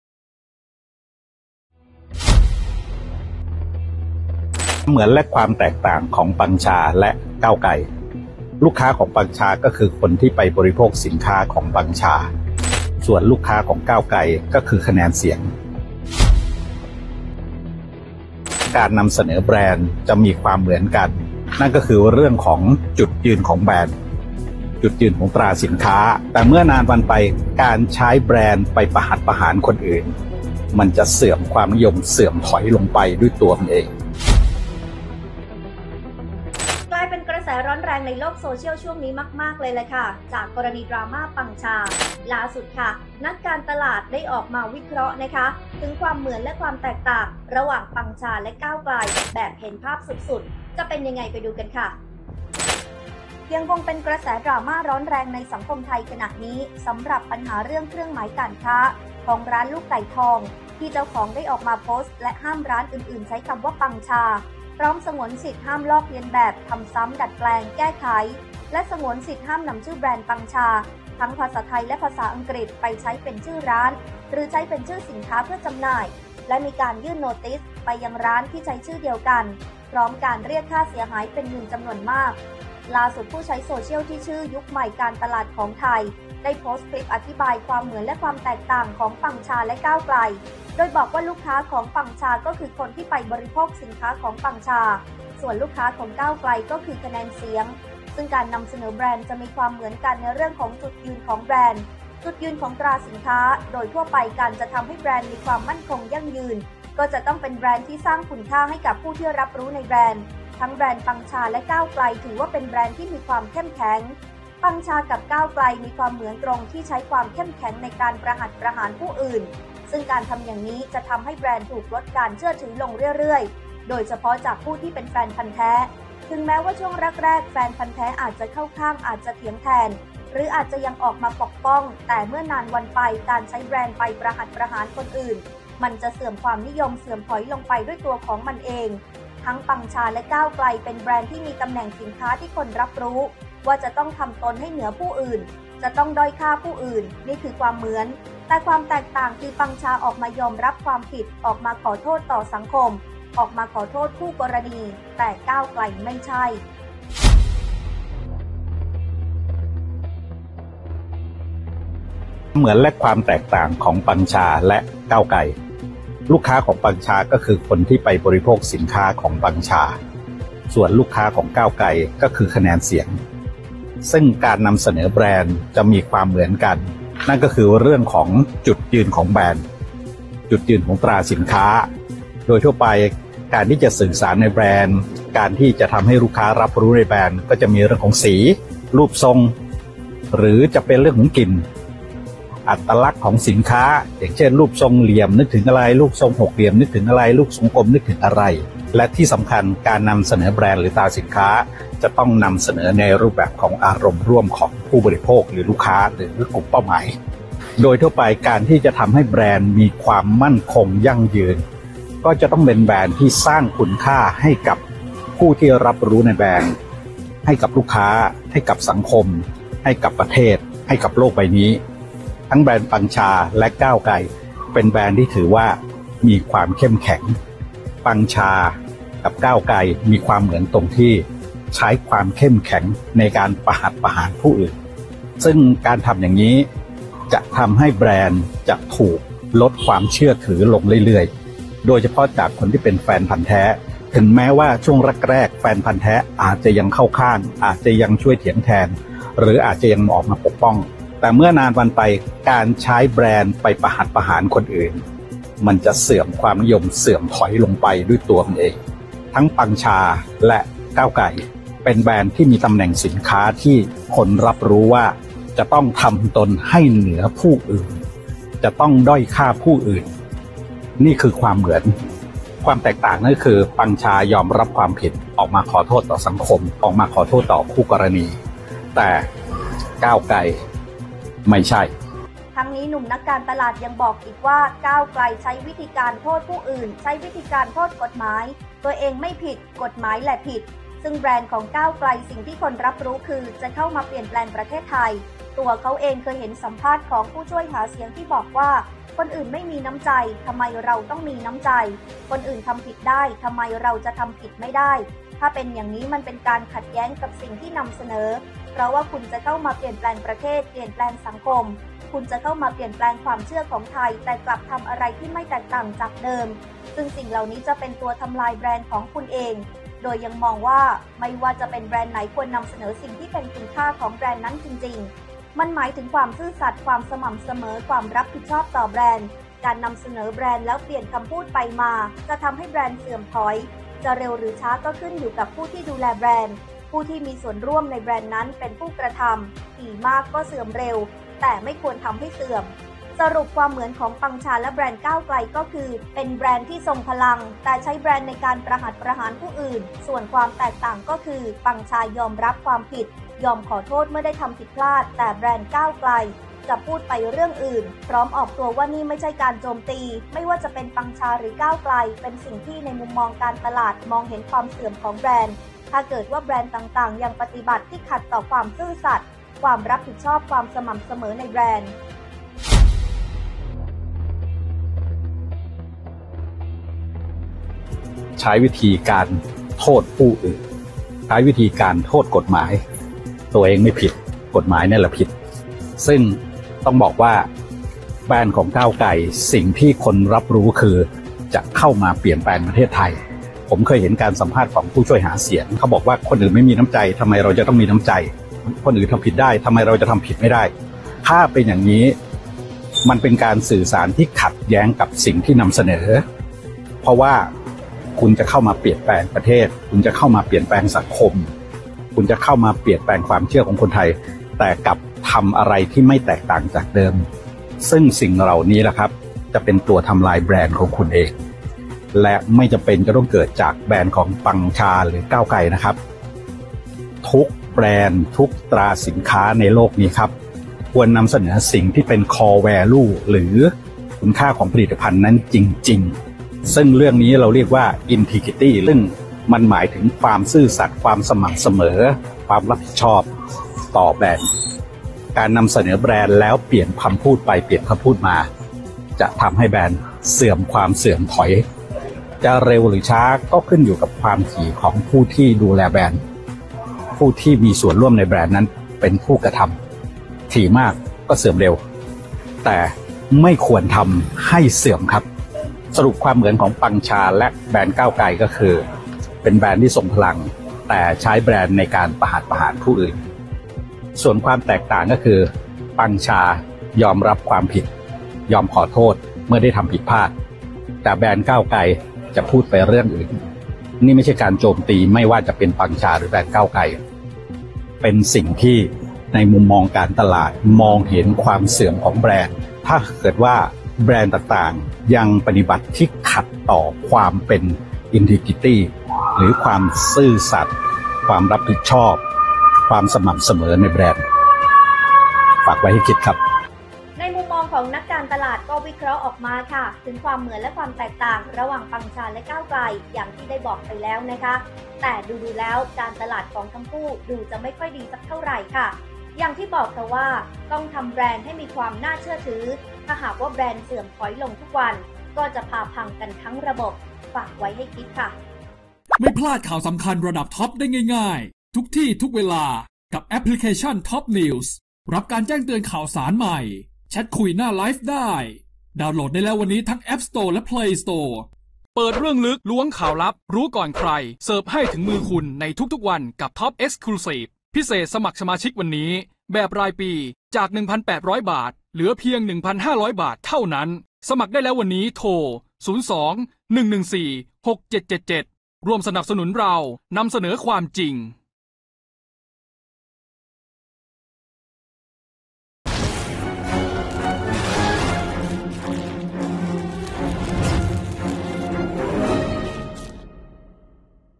“นักการตลาดใหญ่” ชำแหละ ความเหมือนและต่าง “ปังชา-ก้าวไกล” เห็นภาพความพังรำไร |STALKER
เฉียบ “นักการตลาดใหญ่” ถอดรหัสความเหมือนและแตกต่าง ระหว่าง “ปังชา-ก้าวไกล” มองทะลุเห็นความเสื่อมของแบรนด์